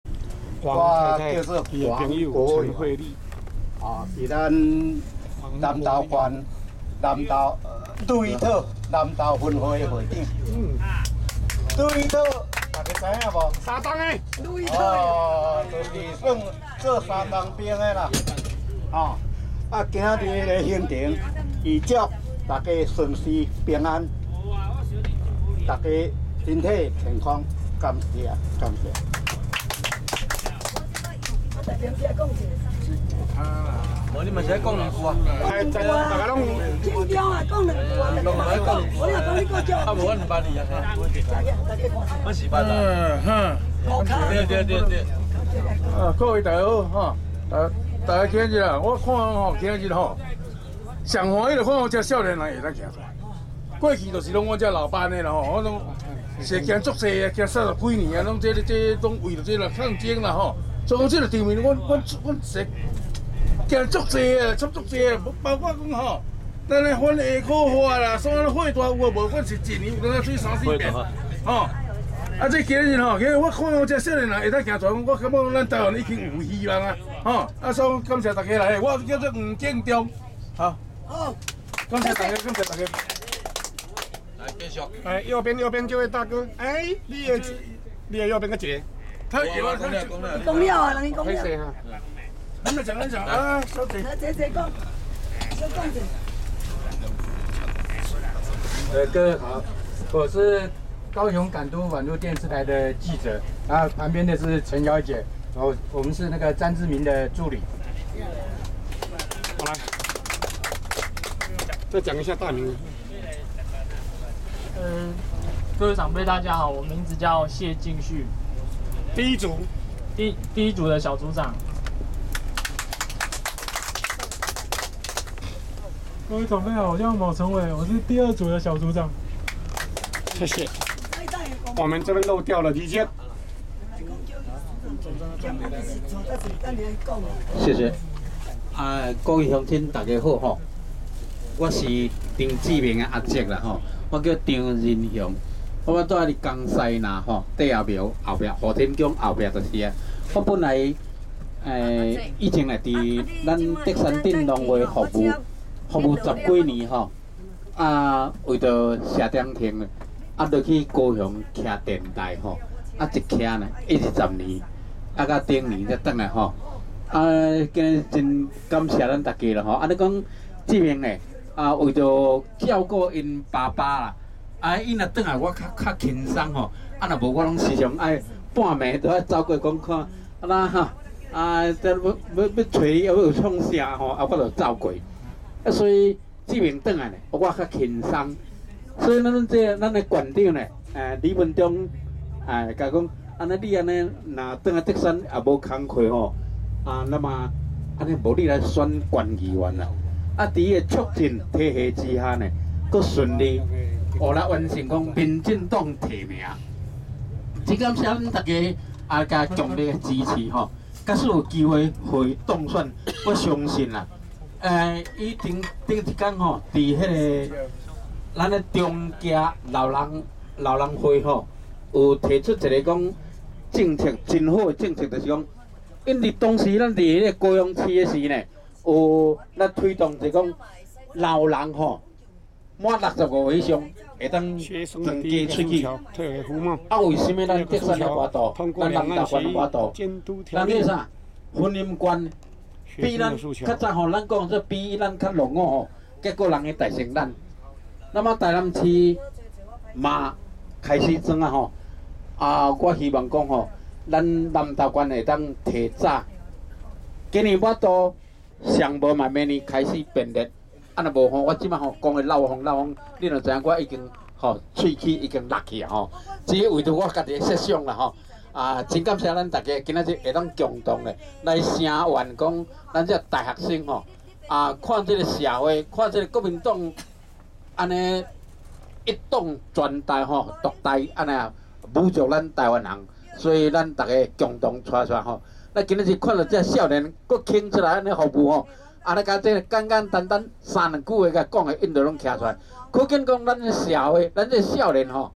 140321竹山 張志銘 動員一車 南投 李文忠前立委動員8車到台北聲援 立法院中的大學生
說明竹山 張志銘 動員一車 南投 李文忠前立委動員8車到台北聲援 立法院中的大學生